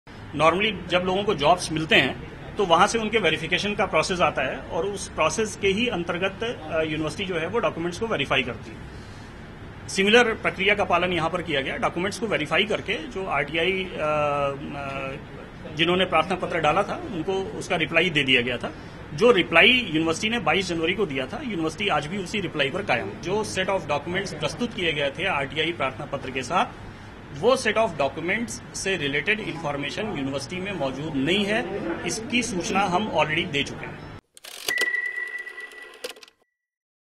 Awadh Univ Says Tomar’s Mark Sheet Doesn’t Match the Roll No
Awadh university clarified that the mark sheet obtained by AAP leader Jitender Singh Tomar does not match the roll number series that was provided.
Video Footage: ANI